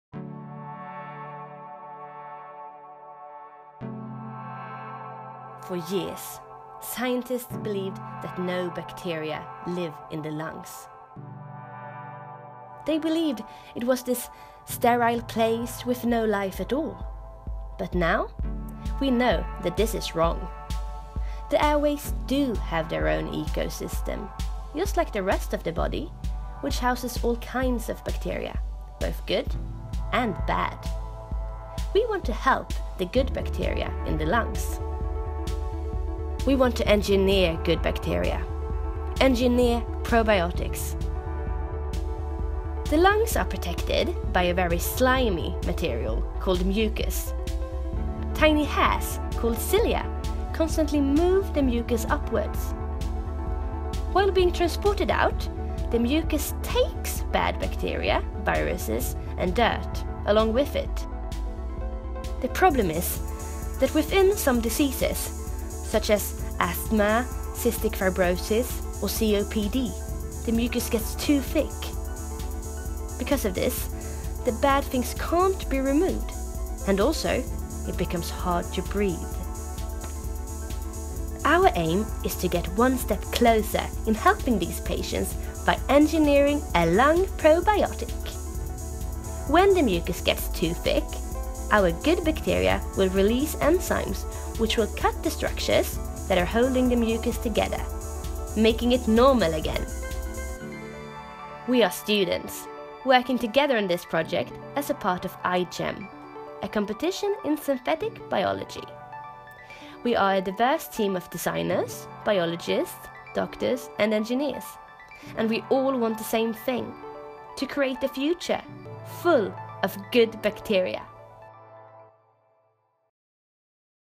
iGEM Stockholm 2017 Project video
We are iGEM Stockholm 2017.
A diverse group of energetic, passionate and open minded people who want to be part of the scientific future.

Our summer was all about science, precisely about re-engineering bacteria and participating in a competition called iGEM.